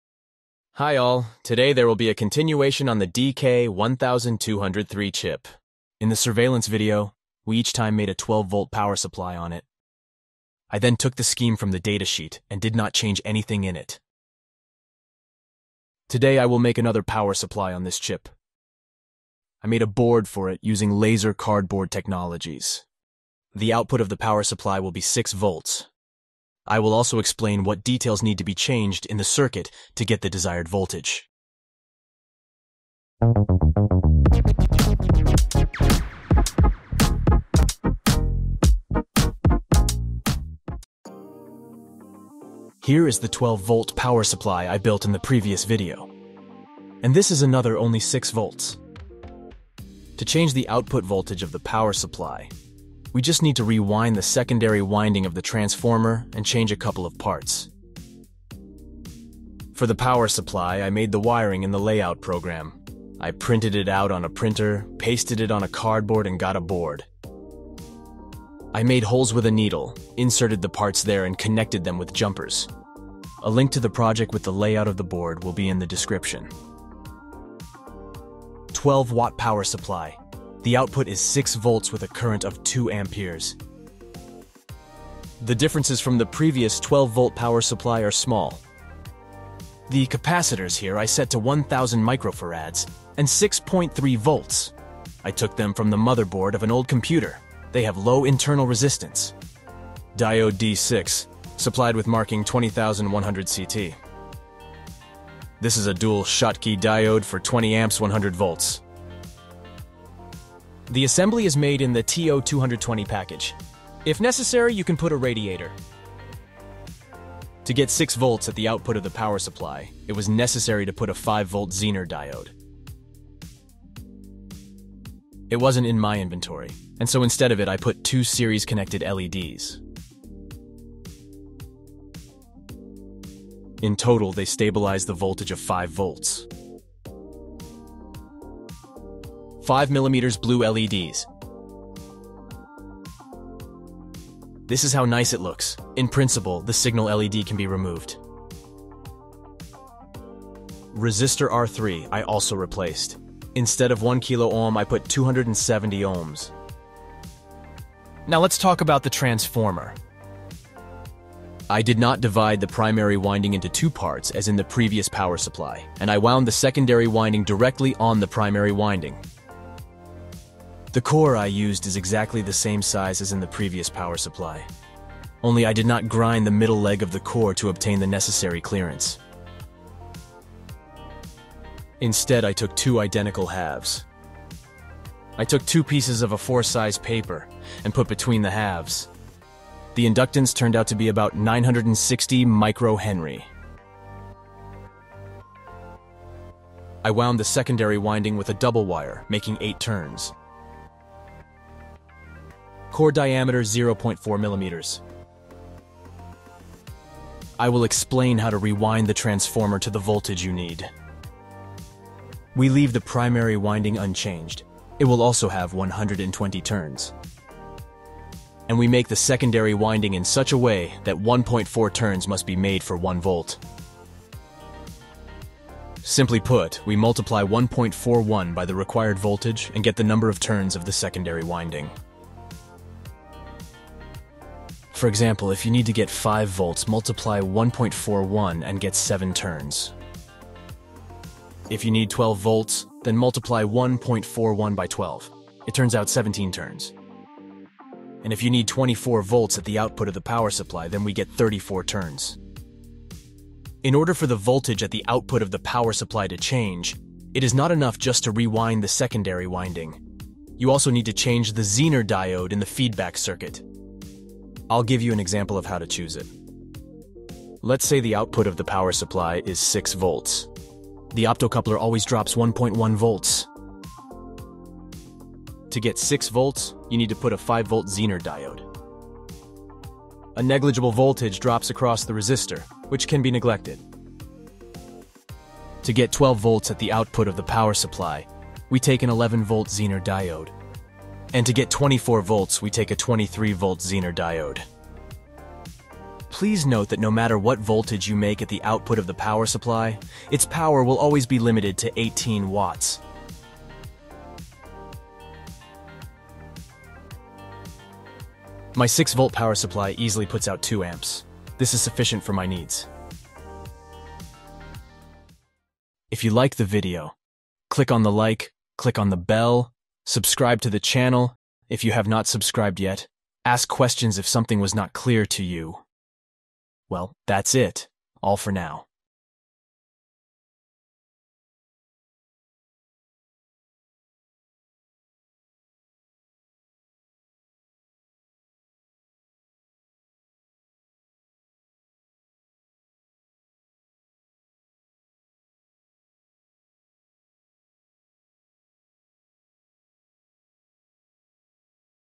Switching power supply on DK1203 for 6 volts (Part 2)
Hello! In this video, I made a continuation of the topic about the DK1203 chip. Last time I assembled a 12 volt switching power supply on it according to the scheme from the datasheet. And today, I will make another power supply, according to the same scheme, but with an output voltage of 6 volts. Also, I will explain what needs to be changed in the circuit in order to get the voltage you want.

Other videos with DK1203 chip
A simple DIY switching power supply on the DK1203: